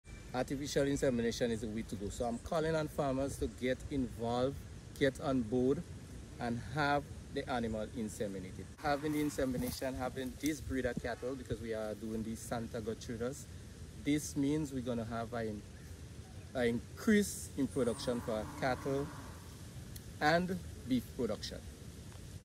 Livestock Assistant Frankie Buckmire weighs in on the upcoming Artificial Insemination Programme
The Ministry of Agriculture and Lands, Fisheries and Cooperatives is initiating moves to resuscitate the cattle industry, to improve genetics and the availability of beef on the local market.
 An ambitious programme is being undertaken with funding from the OECS Regional Agriculture Competitiveness Project (AGRICOM)  and the World Bank’s Food Security Enhancement Project-.

The Ministry plans to resume Artificial Insemination of cattle, having procured sperm from Red Doc farms, a commercially operated farm in the United States, known for supporting breeding programmes and cattle farmers promoting hardiness, heat tolerance and heterosis, as well as carcass quality.

Frankie Buckmire, Livestock Assistant who worked with the past insemination program, is looking forward to its resumption.